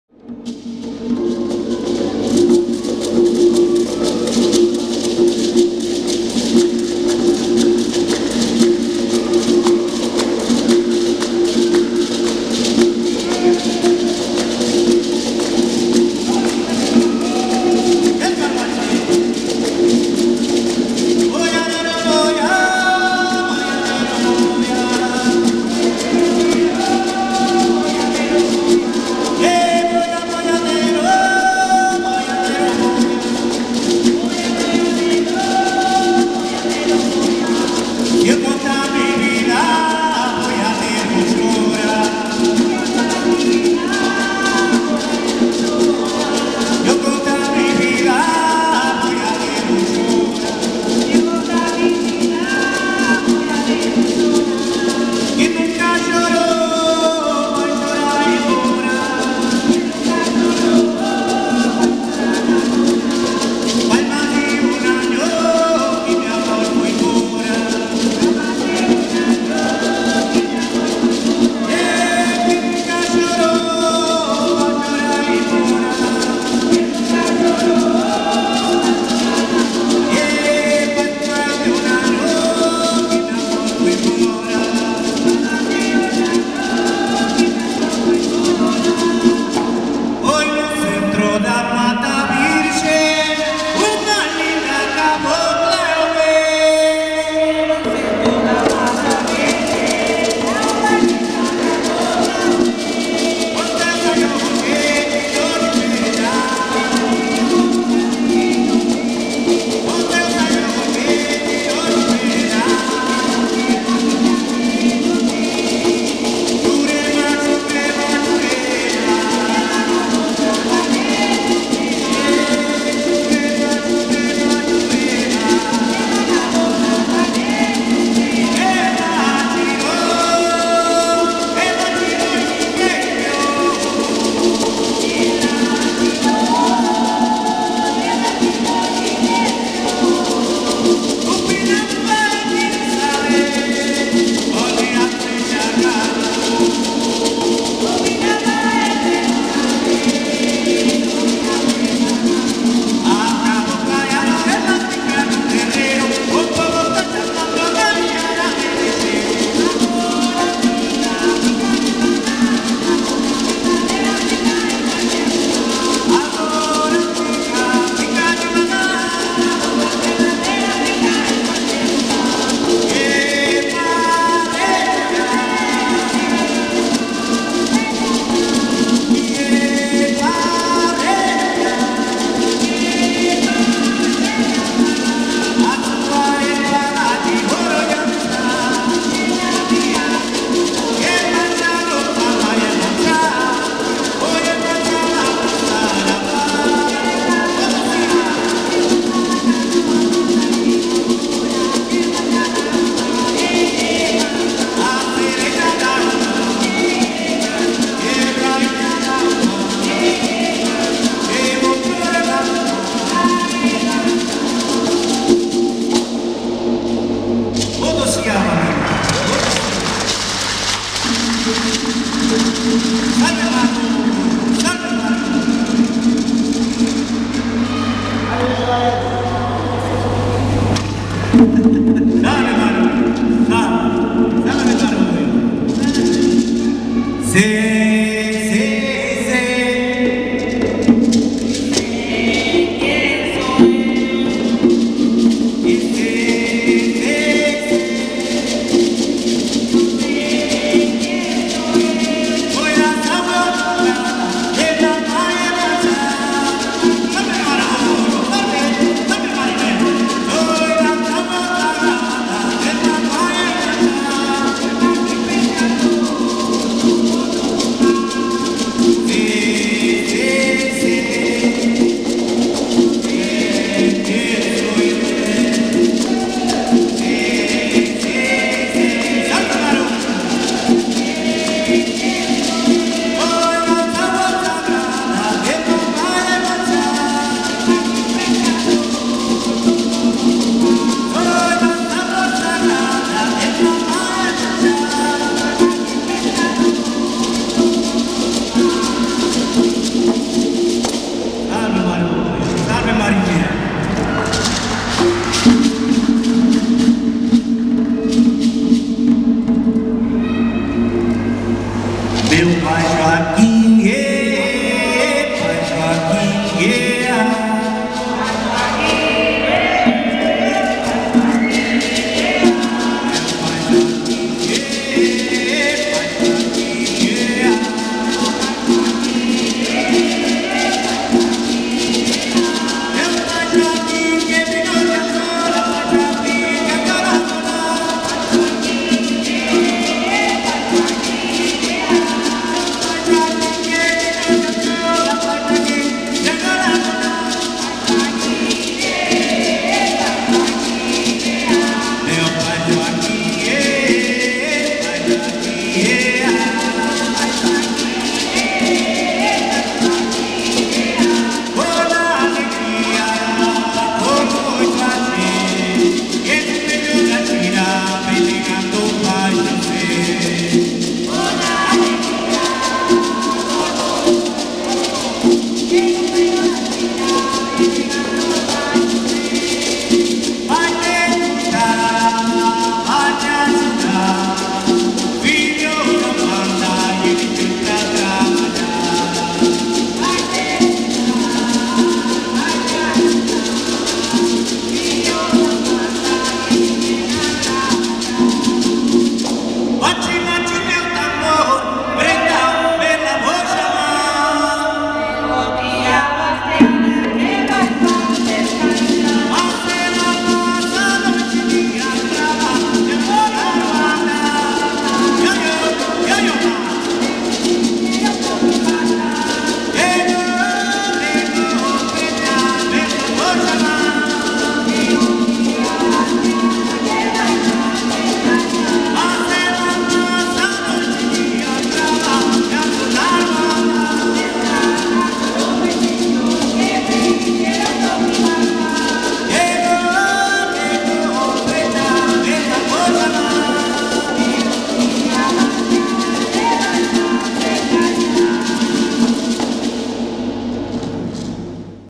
""Aludun un dun Orun"" Tu Fe, Mi Fe,Nuestros Amados Caboclos.
En el Evento de Ezeiza se viste de Blanco, Oloobe Alalemi presento a su Grupo de Percusión, ""Aludun un dun Orun "" Tambores de Ambos Planos"".